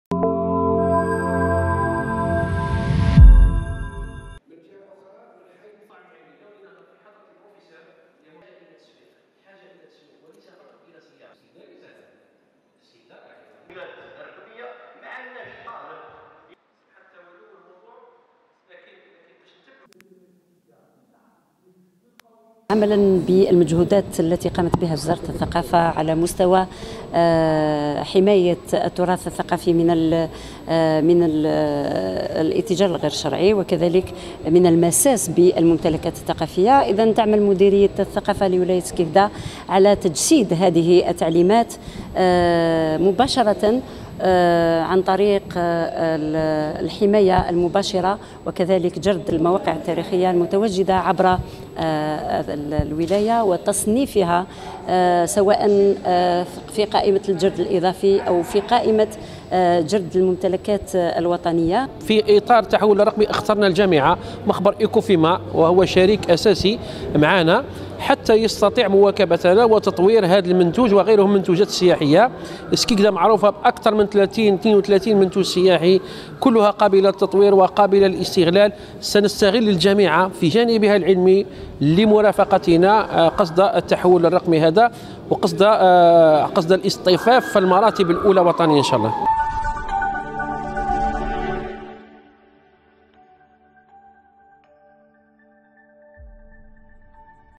سكيكدة : ملتقى وطني للترويج الرقمي للموروث الثقافي والسياحي
سكيكدة (وأج)  نظمت مديرية السياحة والصناعات التقليدية بولاية سكيكدة بالتنسيق مع مديرية الثقافة وجامعة سكيكدة ملتقى علميا للترويج الالكتروني للمعالم السياحية والثقافية بالولاية.